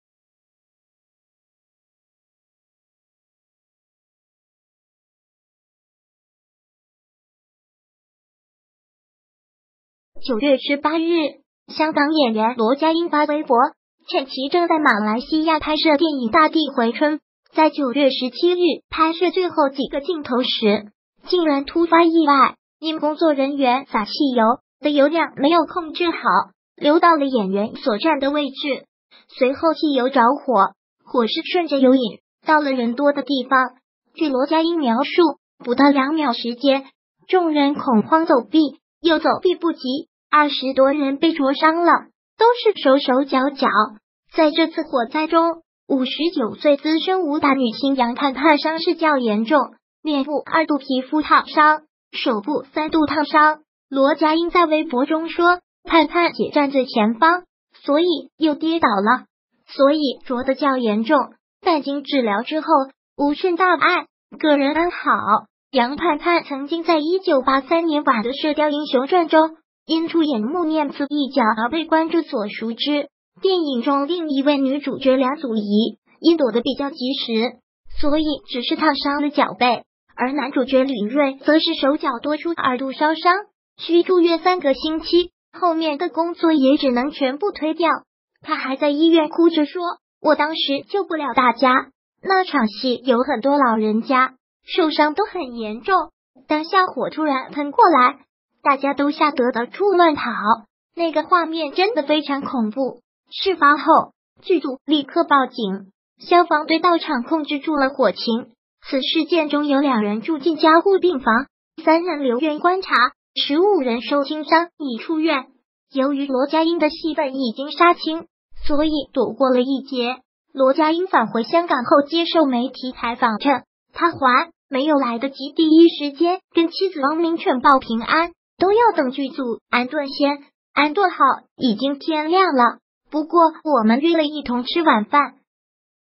片场燃大火，59岁“穆念慈”被三度烧伤，罗家英戏份杀青躲过一劫
9月18日，香港演员罗家英发微博，称其正在马来西亚拍摄电影《大地回春》，在9月17日拍摄最后几个镜头时，竟然突发意外，因工作人员洒汽油的油量没有控制好，流到了演员所站的位置，随后汽油着火，火势顺着油引到了人多的地方。据罗家英描述：“不到两秒时间，众人恐慌走避，有走避不及，二十多人被灼伤了，都是手手脚脚。”在这次火灾中，59岁资深武打女星杨盼盼伤势较严重，脸部二度皮肤烫伤，手部三度烫伤。罗家英在微博中说：“盼盼姐站最前方，所以又跌倒了，所以灼得较严重，但经治疗之后，无甚大碍，各人安好。”杨盼盼曾经在1983年版的《射雕英雄传》中，因出演穆念慈一角而被观众所熟知。电影中另一位女主角梁祖仪因躲得比较及时，所以只是烫伤了脚背，而男主角吕锐则是手脚多处二度烧伤，需住院三个星期，后面的工作也只能全部推掉。他还在医院哭着说：“我当时救不了大家，那场戏有很多老人家，受伤都很严重。当下火突然喷过来，大家都吓得到处乱跑，那个画面真的非常恐怖。”事发后，剧组立刻报警，消防队到场控制住了火情。此事件中有两人住进加护病房、13人留院观察、15人受轻伤已出院。由于罗家英的戏份已经杀青，所以躲过了一劫。罗家英返回香